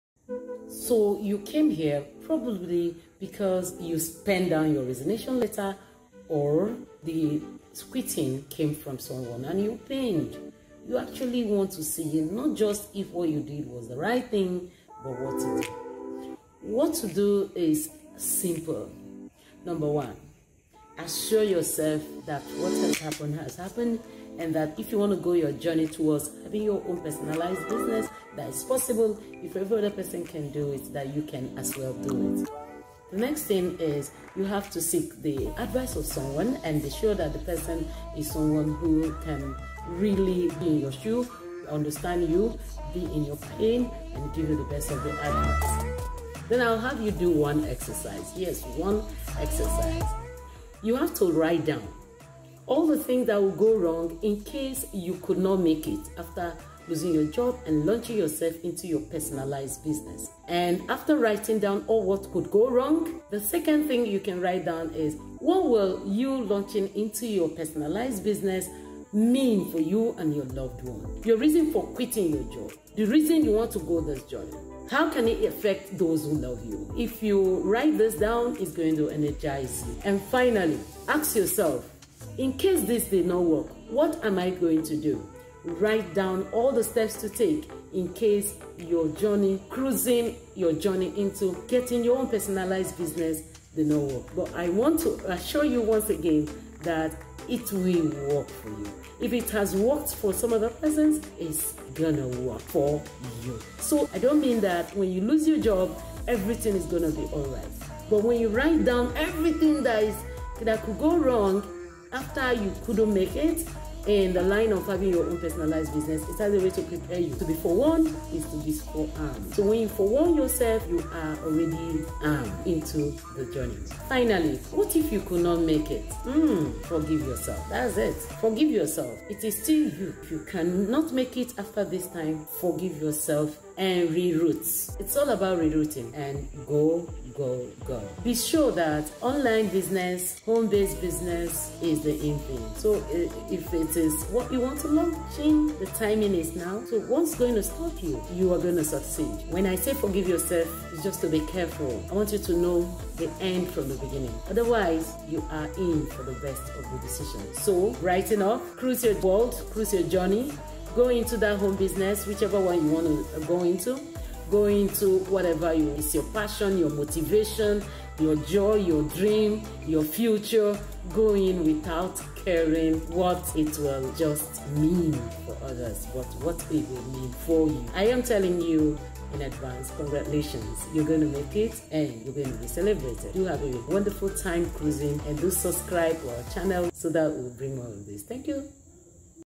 WHAT TO DO WHEN YOU LOSE YOUR JOB
When you are scared of how to switch to your dream home based business in the online space, this is the video for you.

You need something that can give you that extra push to own your life, yes take back your life and move to your dream land.

Start that home business today.
You can do this.
No one can do it for you